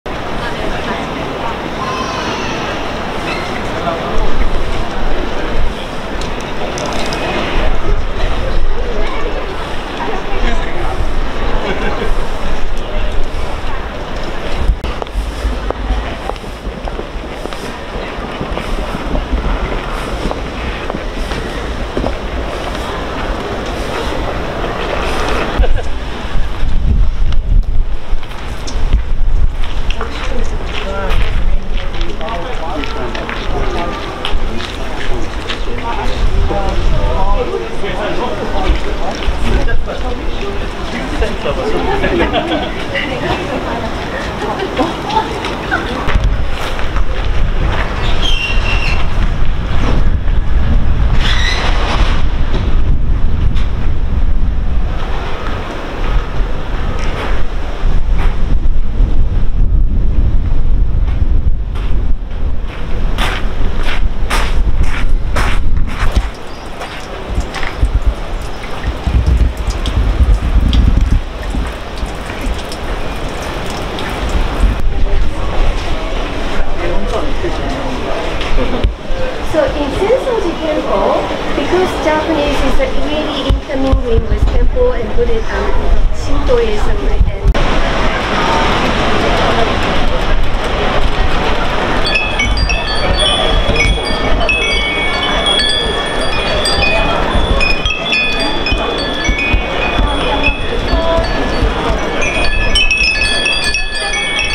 三社祭2024 開催迫る 浅草は祭り一色へ
三社祭まで1週間。浅草は既に祭り一色に染まっています。